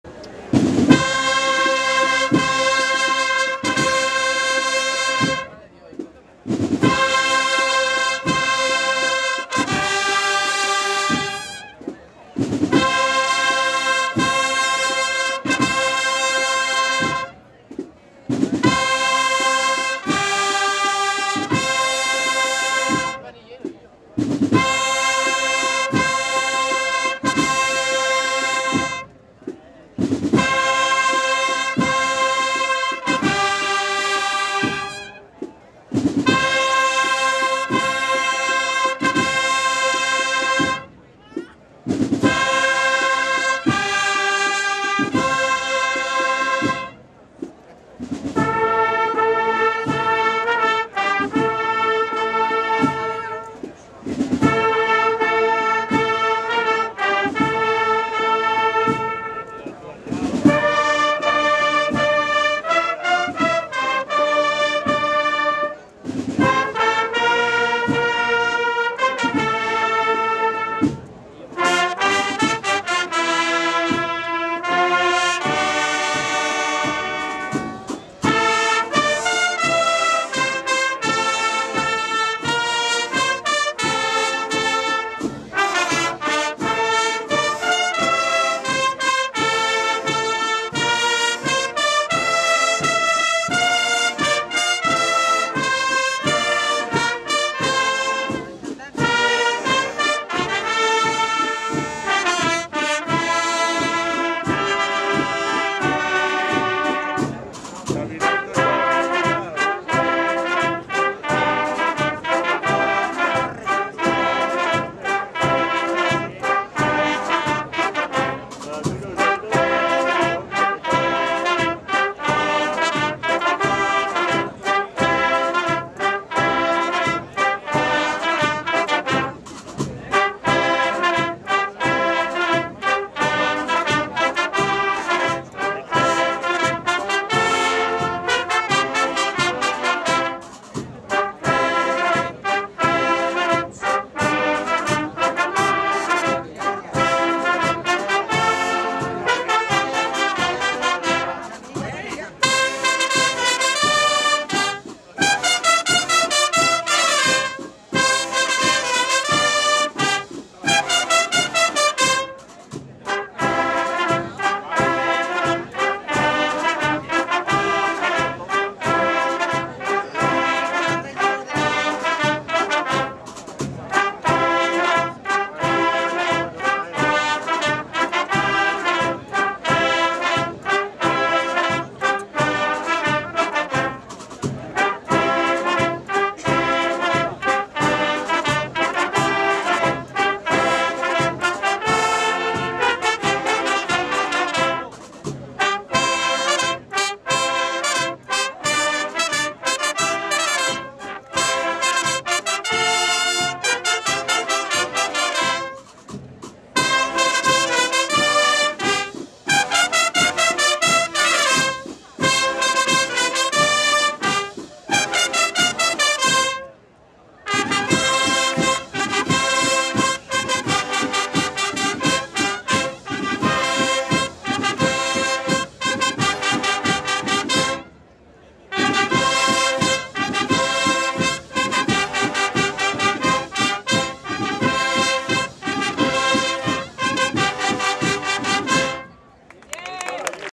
V. de los Reyes en expocofrade 2009 4
Actuación de esta banda en las fiestas del rosario de la macarena